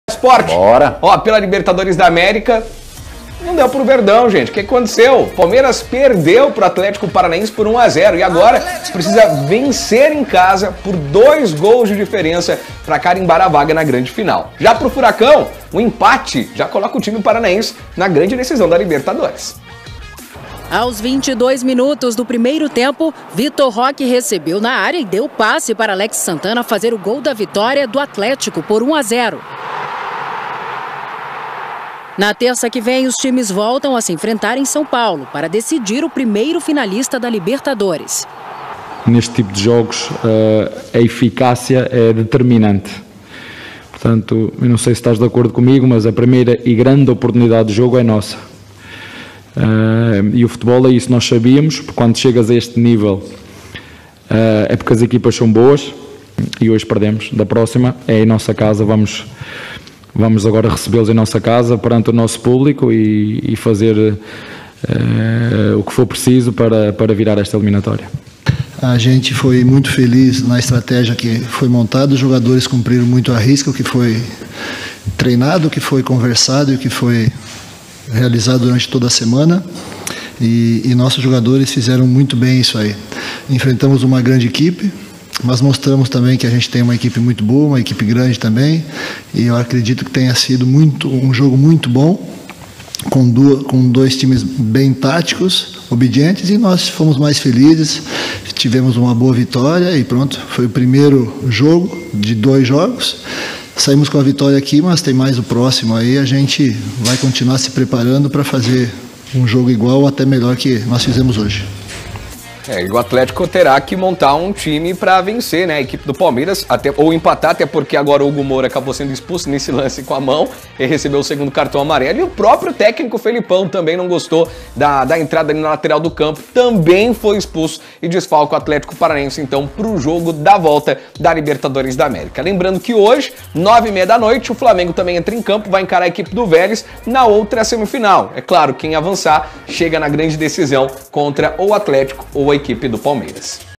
Atlético vence o Palmeiras no primeiro jogo da semifinal
Com um jogador a menos, o Atlético venceu o Palmeiras no primeiro jogo da semifinal.